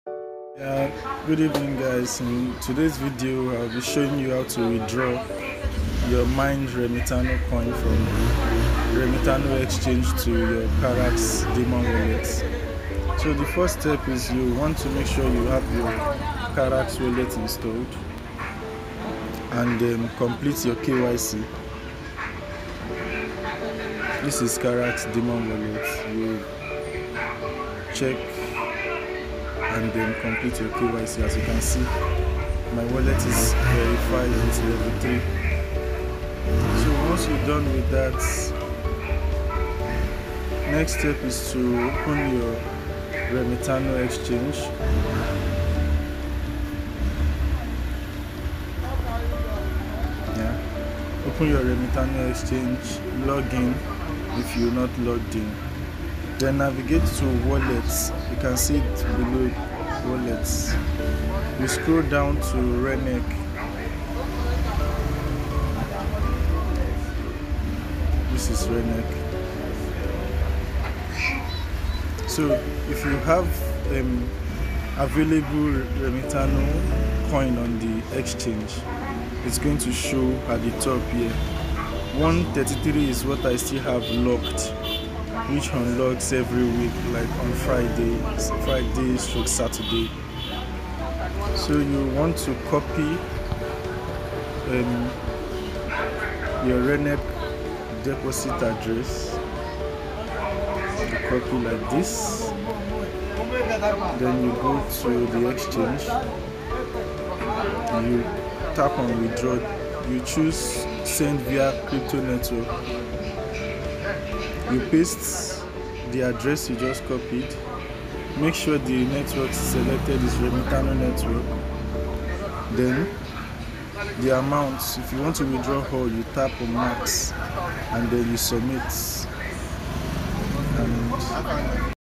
How To Withdraw RENEC from Remitano Exchange To Carax Demon Wallet
Withdrawing Your Mined RENEC From Remitano Exchange To Carax Demon Wallet
Step 1 - Download Carax Wallet
Step 2 - Complete KYC
Step 3 - Copy RENEC Deposit Address
Step 4 - Open Your Remitano Exchange
Steo 5 - Navigate To Withdrawal Tab
Steo 6 - Paste RENEC Address, Select Network
& Authorize Withdrawal